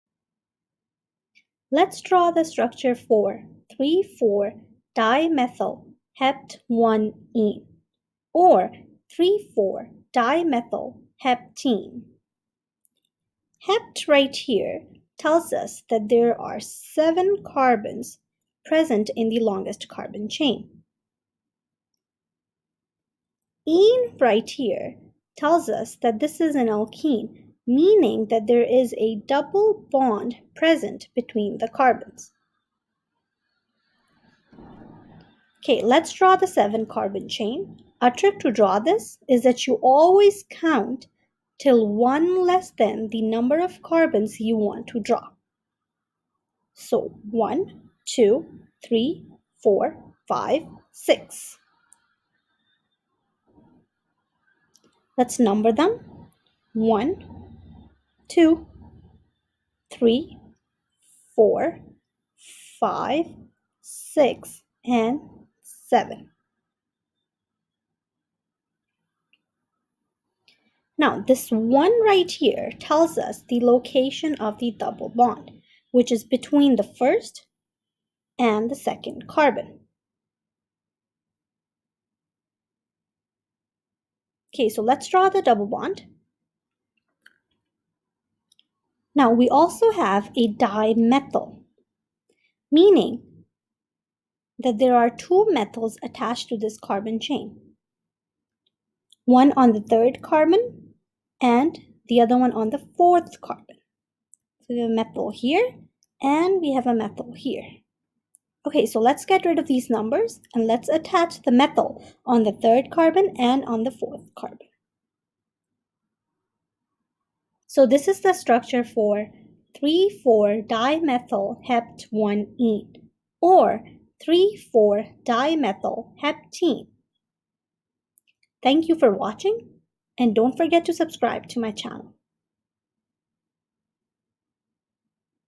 How to draw the structure for 3,4-dimethylhept-1-ene | Drawing Alkenes | Organic Chemistry
How to draw the structure for 3,4-dimethylhept-1-ene | Drawing Alkenes |
Organic Chemistry Introduction

Drawing structures for alkenes in a step by step tutorial

Learn to draw the structure for  3,4-dimethylhept-1-ene or 3,4-dimethylheptene in an easy way!

To write the structure for  3,4-dimethylhept-1-ene or 3,4-dimethylheptene, we first draw the longest carbon chain with 7 carbons. Then we number the carbons and add another bond (double bond) between the 1st and the 2nd carbon. Then we add a methyl on the 3rd carbon and on the 4th carbon.